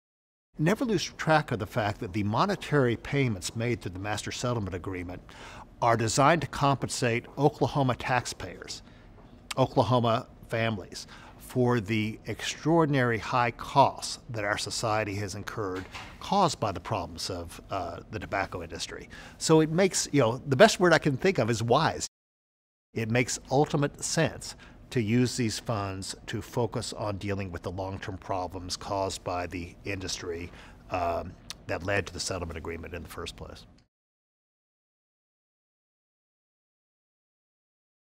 Keeping the Promise for Health - Robert Butkin
“It makes ultimate sense to use these funds to focus on dealing with the long-term problems caused by the (tobacco) industry that lead to the settlement in the first place.” - Robert Butkin, State Treasurer of Oklahoma, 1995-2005.

Oklahoma was the first state in the nation to constitutionally protect its Master Settlement Agreement payments. Learn more about the history and continued impact of TSET here: TSET.ok.gov.